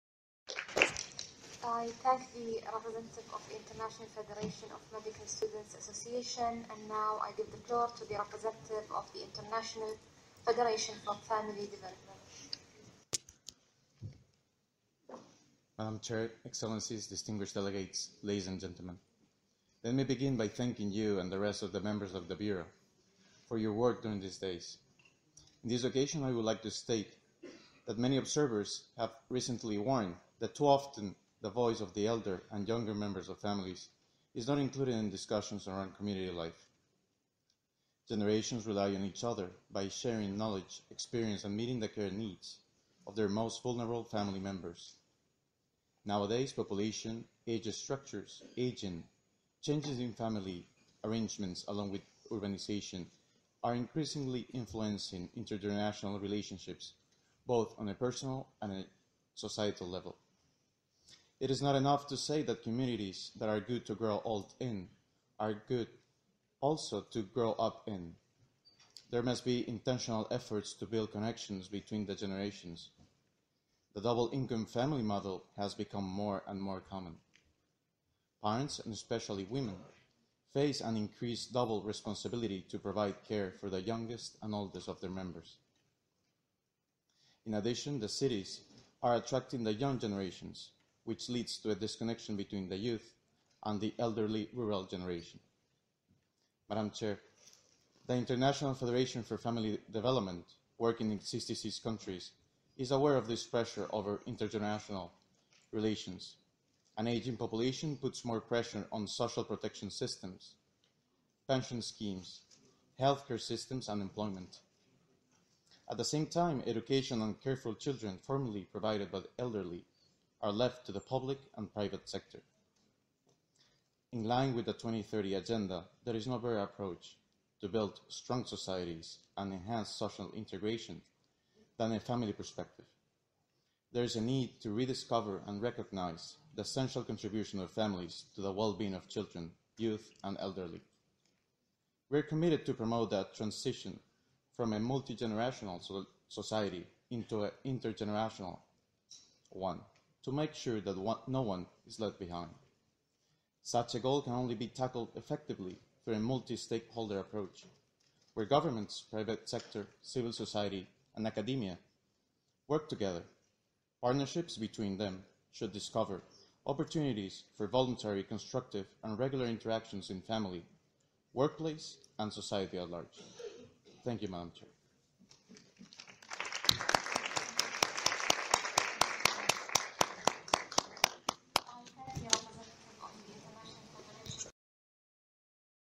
IFFD Oral Statement at CPD50
Oral Statement made by Alex Vazquez during the Commission on Population and Development on behalf of IFFD (6 April 2017).